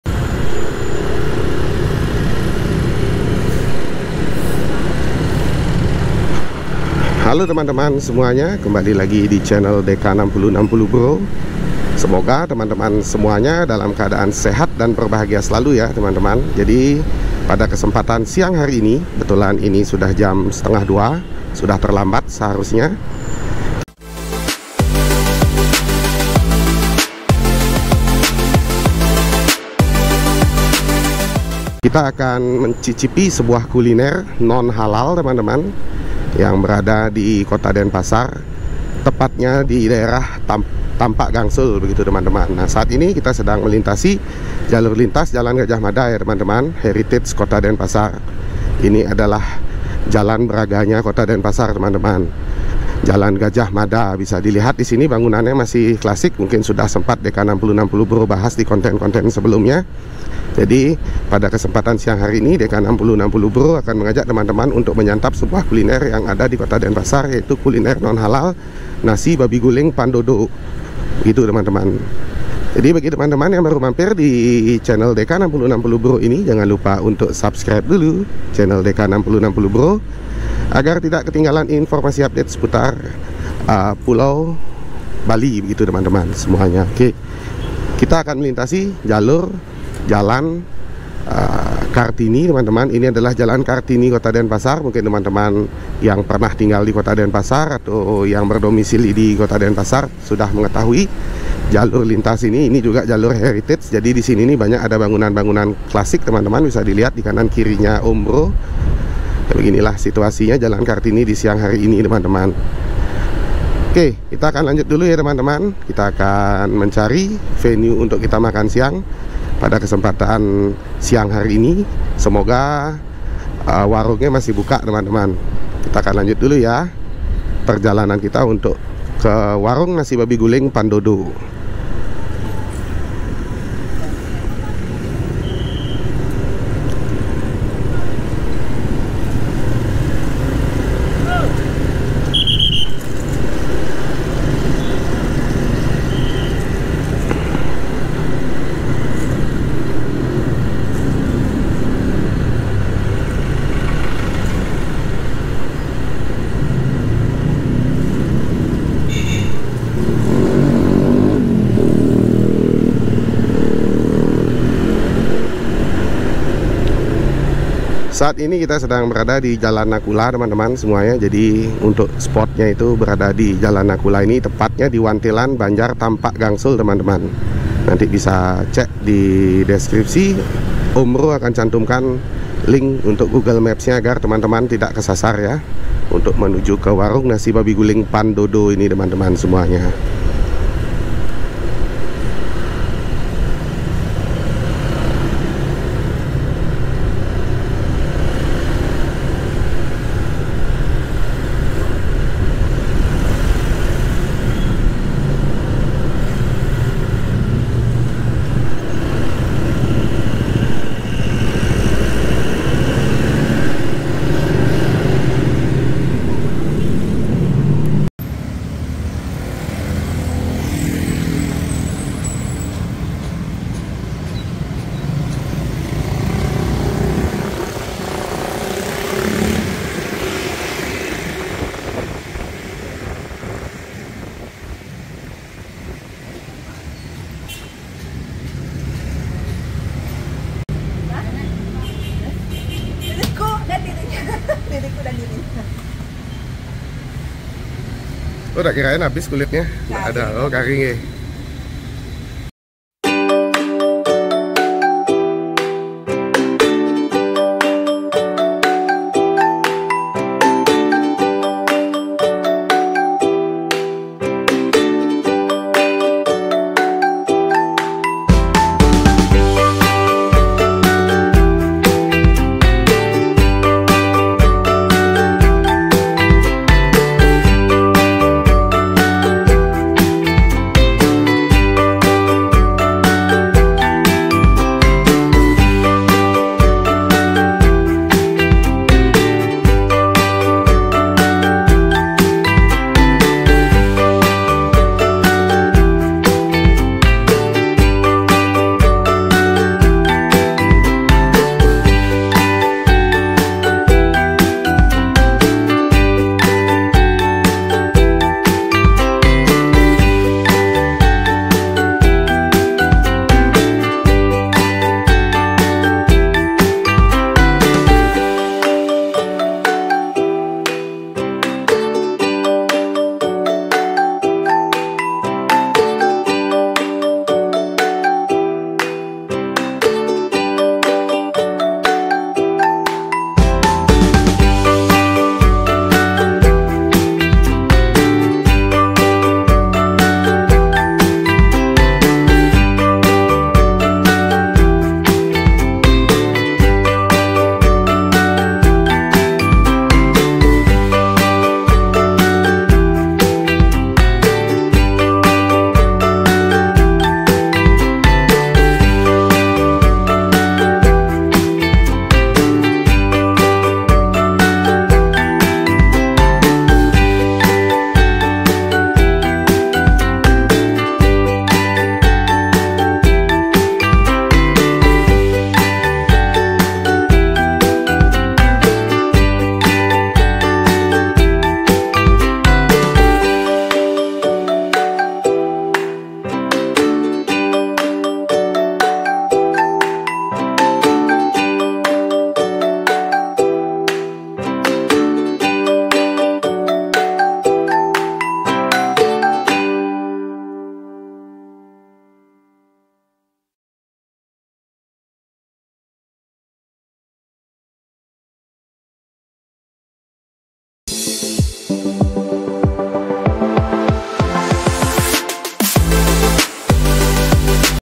KULINERIDE | Rekomendasi Kuliner Nasi Babi Guling Khas Bali di Kota Denpasar | Babi Guling Dodo
Halo teman² semua, DK 6060 BRO akan memberikan info rekomendasi Tempat Makan Siang Kuliner Khas Bali (Non Halal) Nasi Babi Guling di Kota Denpasar yaitu

Nasi Babi Guling Dodo
Ini Lokasinya :

Salah Rekomendasi Warung Makan Kuliner Khas Bali Nasi Babi Guling Yang ada di Kota Denpasar yang pas untuk Makan Siang.